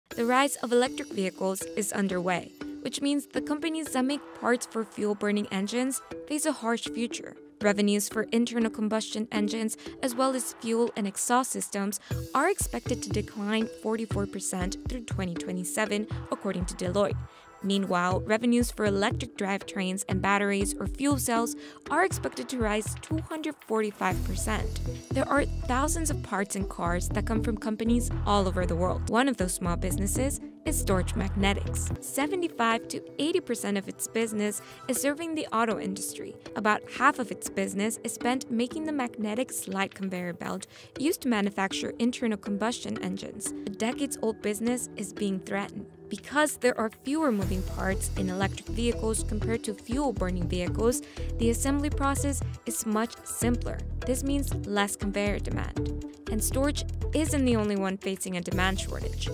How electric vehicles are upending the 100-year-old auto supply chain
Companies that make parts for internal combustion engines are facing a harsh future. Revenues for ICE cars, and fuel and exhaust systems are all expected to decline 44% through 2027. While the parts themselves are changing, the pie is also shrinking. Qill EVs kill the thousands of auto suppliers or can they find ways to pivot, consolidate or somehow stay in business?